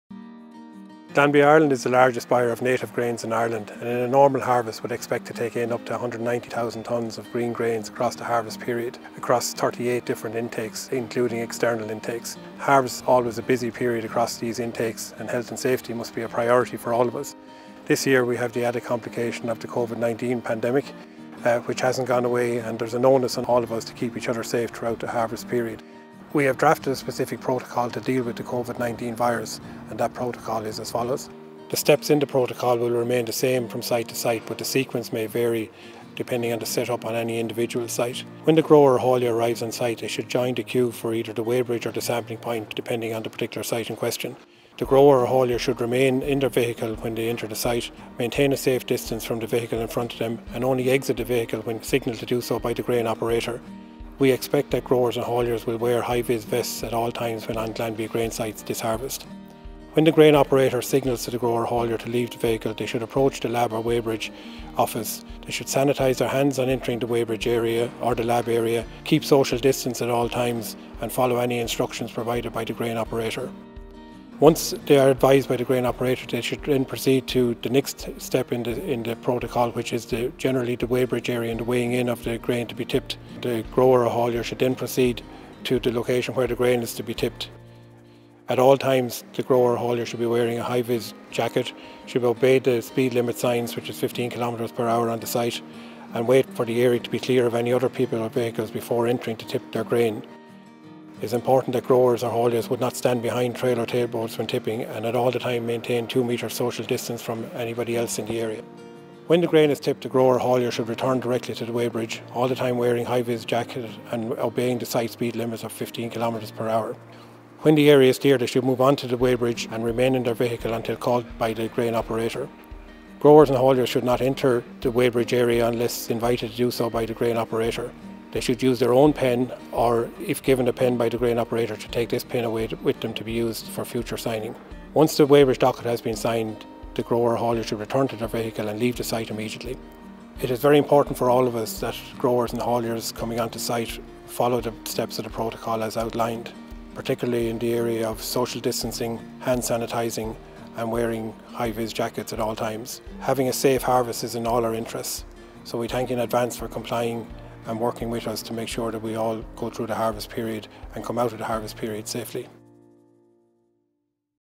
Grain Delivery Protocols
Hear from Donal Moloney, Glanbia Ireland, Grain Manager as he discusses the protocols involved for delivering grains during the harvesting season.